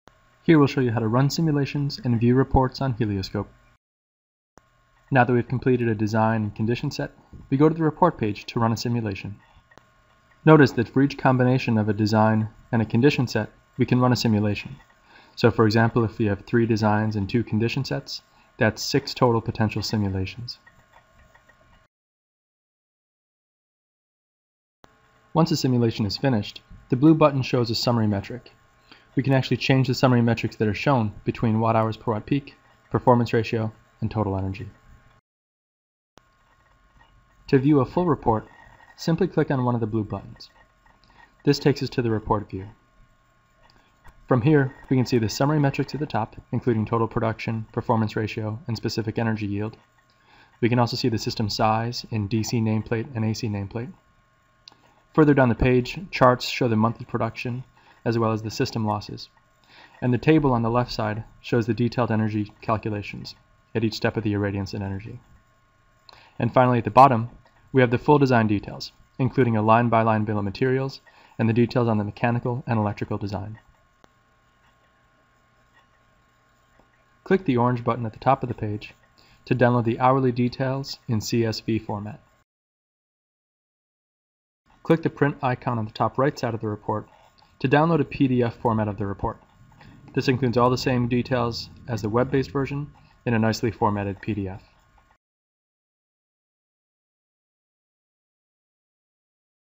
Run Simulation & View Reports in HelioScope
This training video describes how to run a Simulation and manage the resulting Reports in HelioScope, the solar design & modeling software from Folsom Labs.